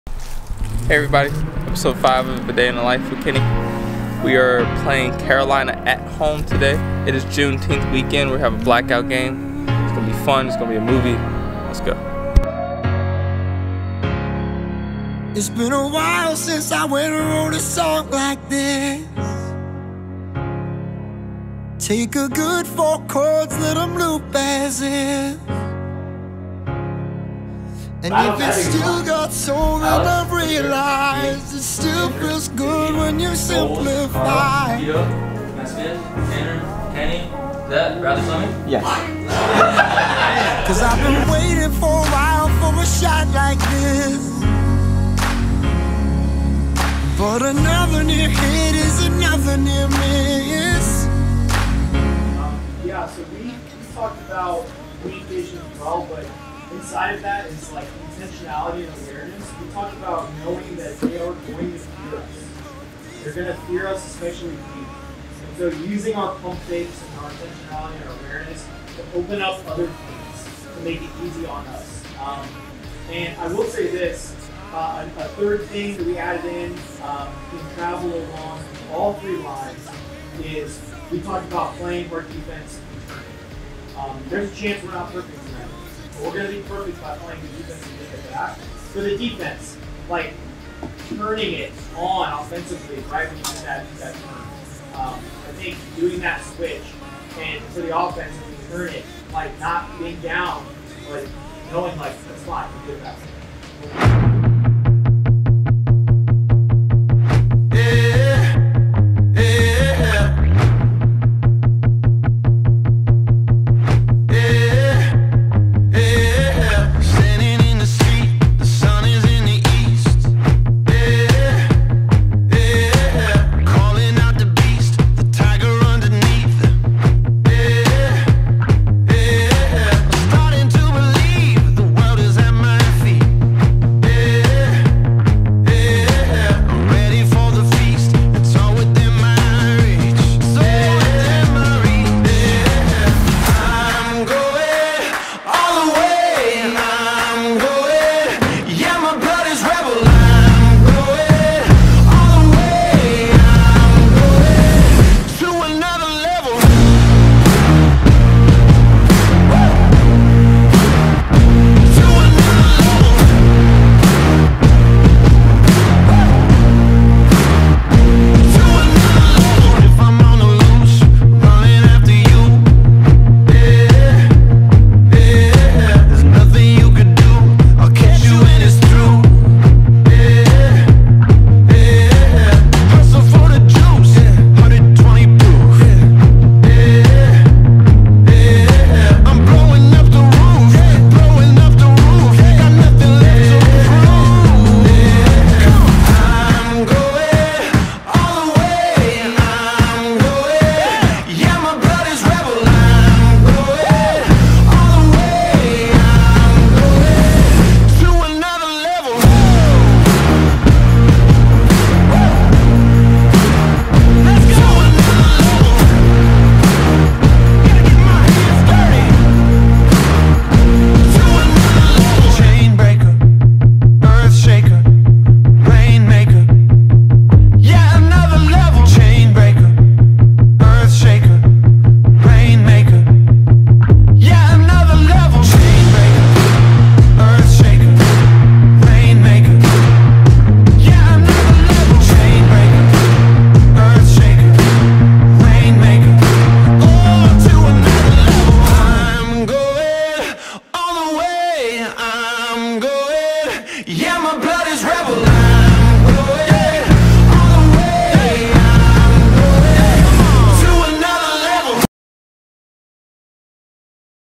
VLOG 5
Come with me as go through Week 8 of the AUDL. ATLANTA HUSTLE VS CAROLINA FLYERS @ HOME